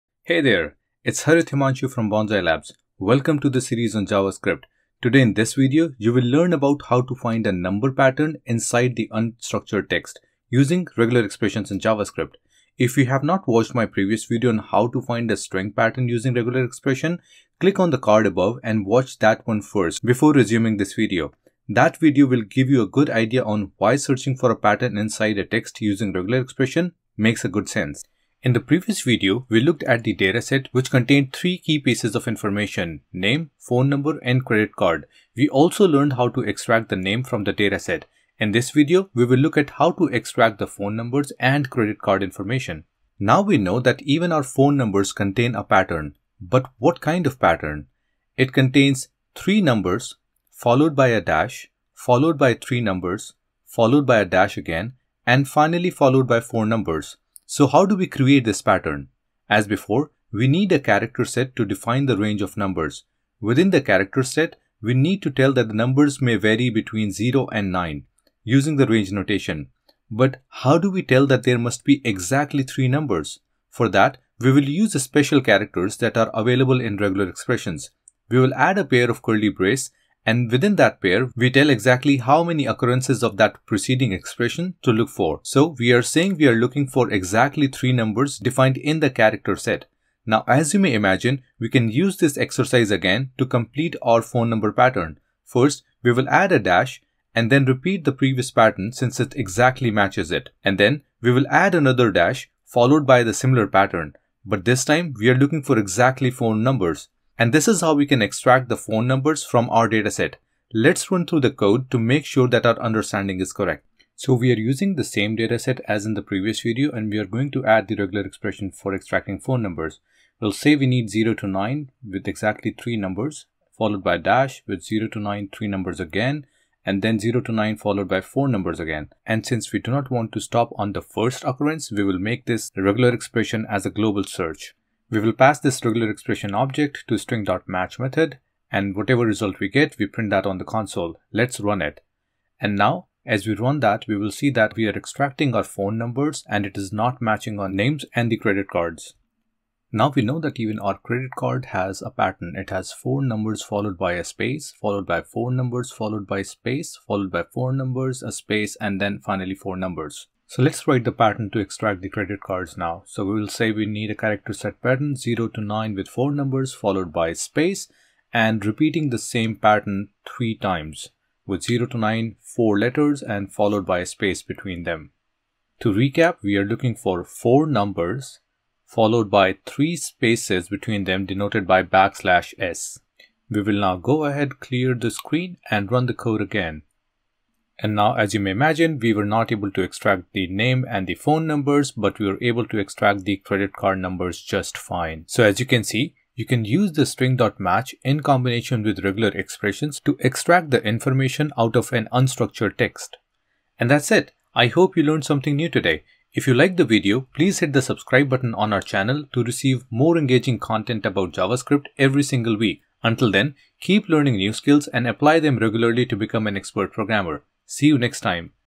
Matching Numbers in JavaScript Strings using Regular Expressions
Learn how to match number patterns such as phone numbers and credit cards in the unstructured text using JavaScript Strings
This video is a follow-up video for - Search a String by matching against a regex in JavaScript available

In this video, we will extract phone numbers and credit card data using regular expression and String.match available in JavaScript

Watch all JavaScript Tutorials